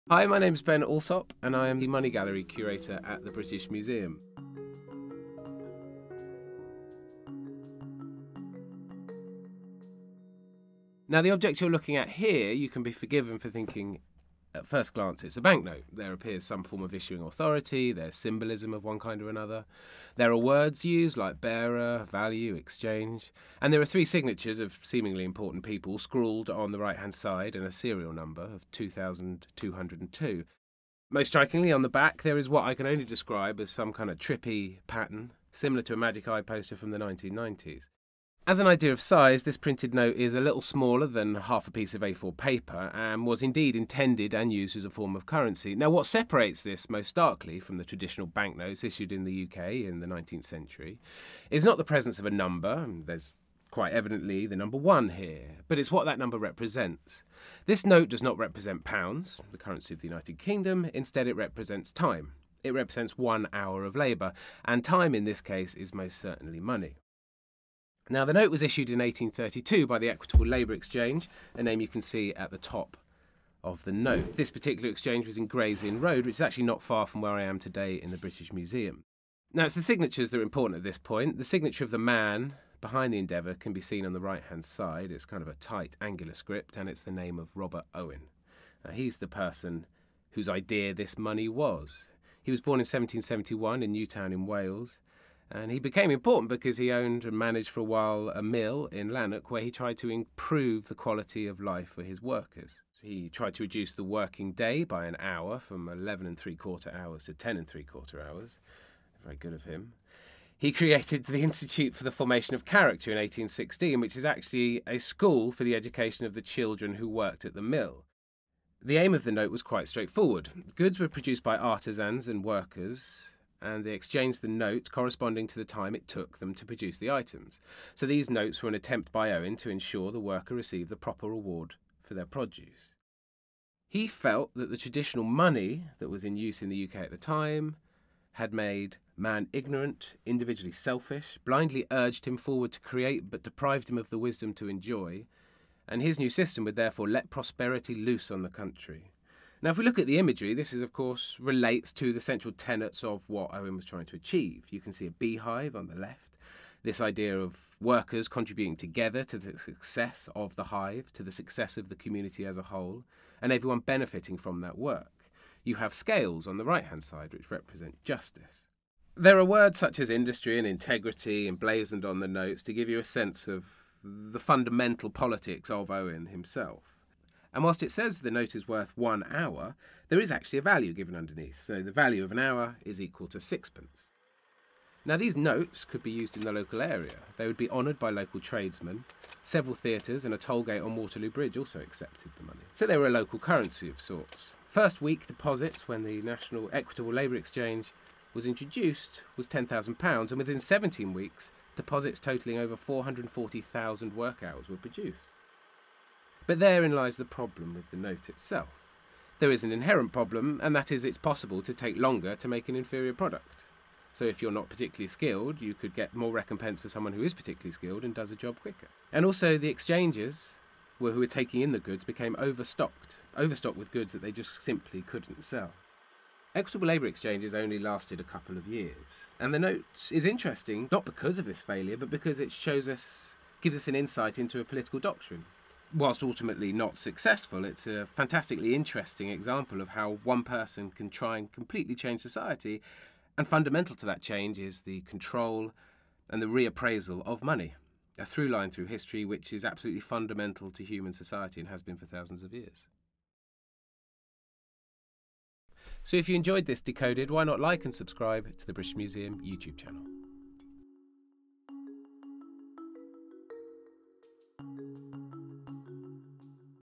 Time-Based Currency by Robert Owen | DECODED #4
Curator Ben Alsop explores a different kind of economic world, where wages and prices are measured in hours instead of units of precious metal. The idea came from Robert Owen, one of the founders of utopian socialism and the cooperative movement. He's achievements included development of cooperatives and the trade union movement and supporting the passage of child labour laws and free co-educational schools. His principles were radical for the time, and many of his achievements are things we still depend on today. Sadly, because humans will always find a way to cheat a system, his time-based currency is not one of those achievements.